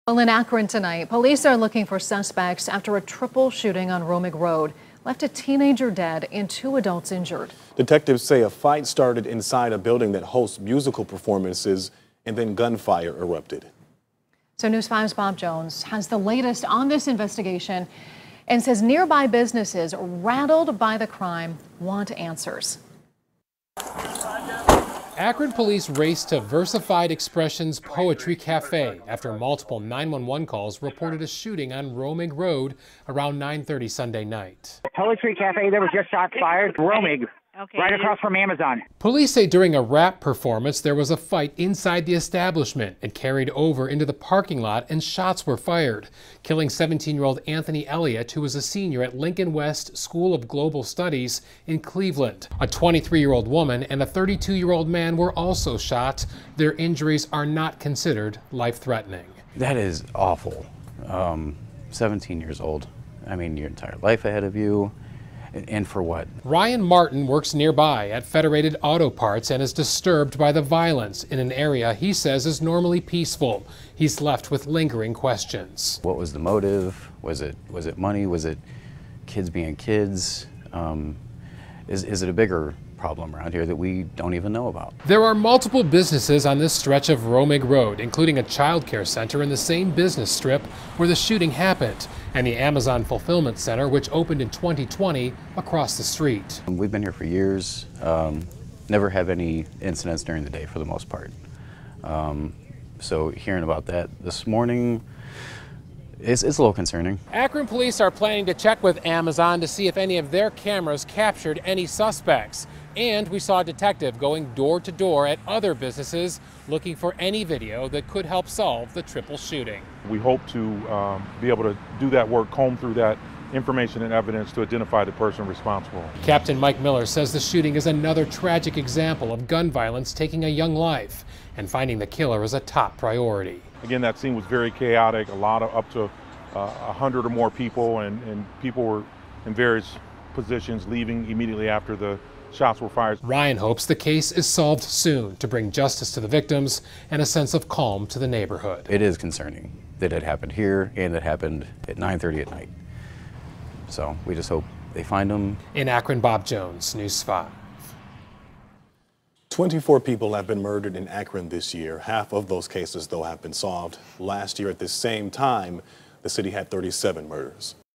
17-year-old boy killed, 2 others shot in Akron
The Akron Police Department is investigating a shooting that left a 17-year-old boy dead. The shooting happened Sunday night.